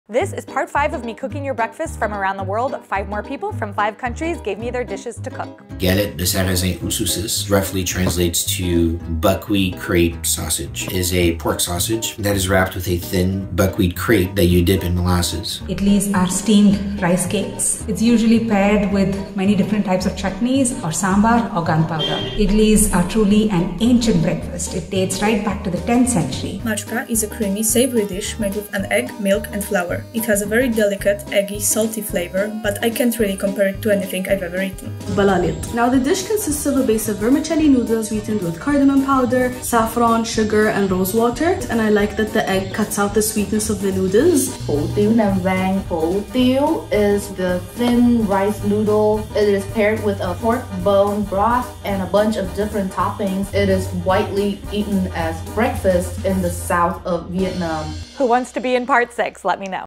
I Didn't Know People Ate Breakfast Like This@!
Thank you so much to Florek, Thien, Louis, Miriam, and Sangeetha for sharing your favorite dishes with us!

Wanna mail something?
Beryl Shereshewsky
115 East 34th Street FRNT 1